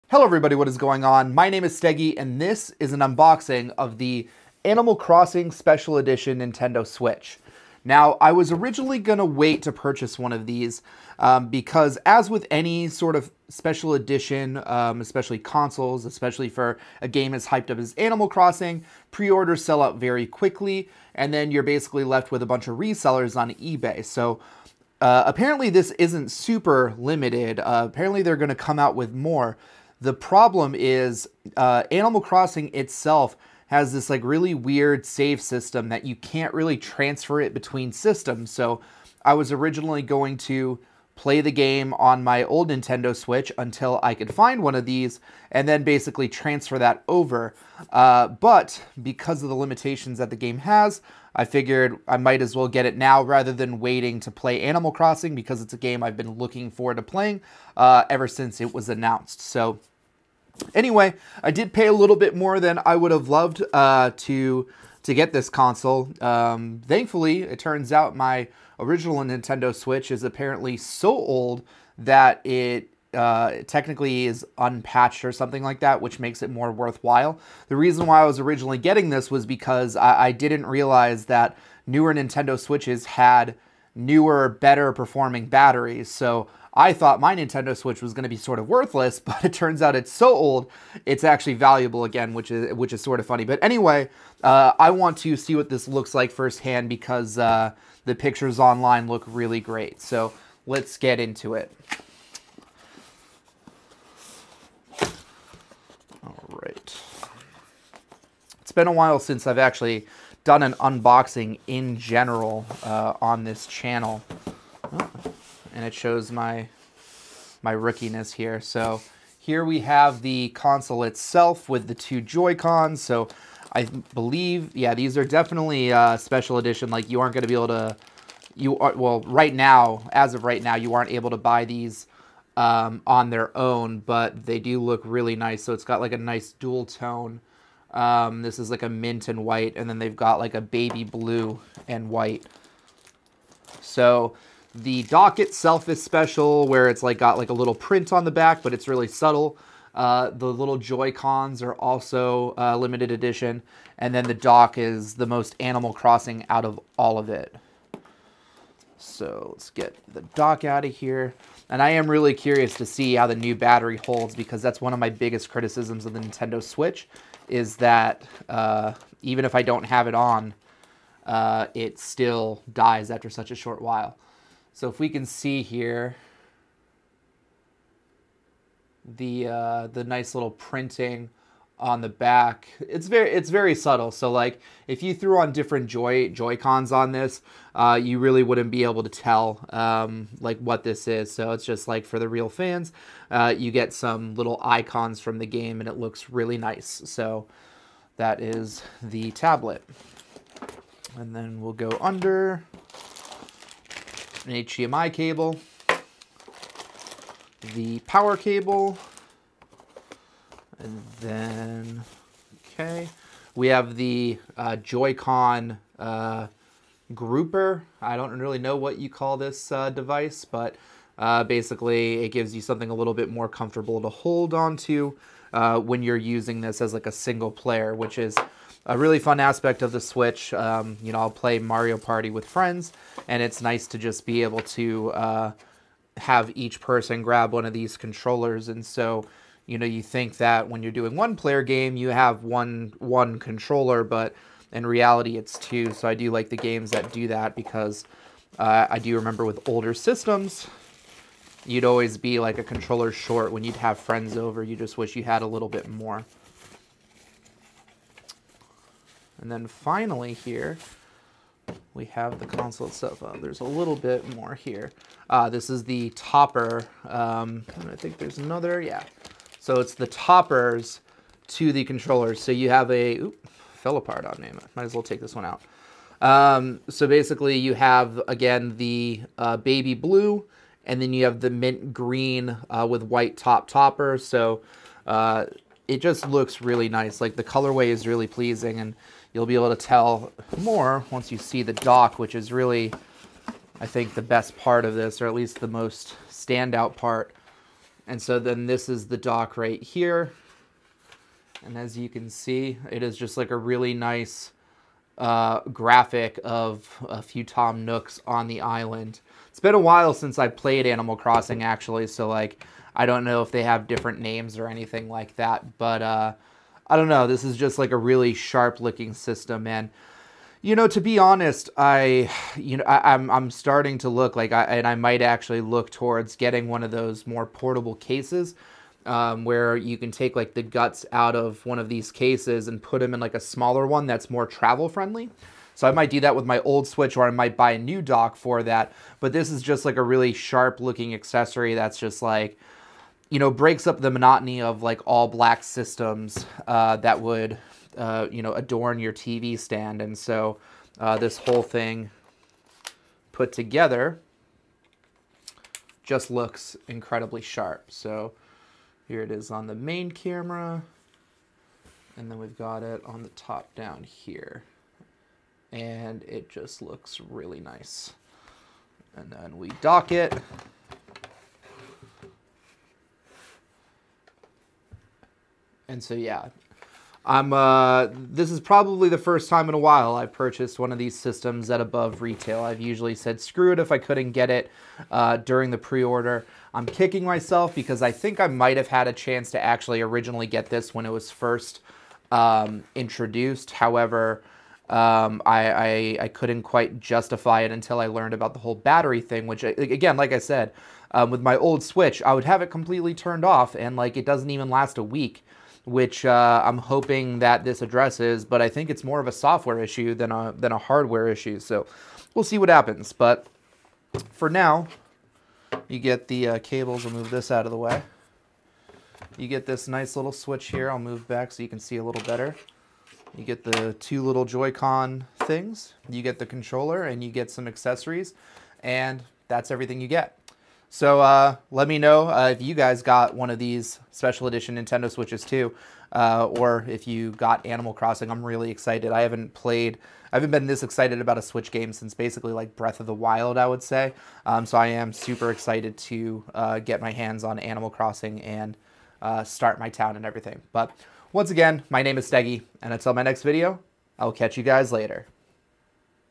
Animal Crossing Special Edition Nintendo Switch Unboxing!
This is an awesome limited edition Nintendo Switch, come check out what's inside.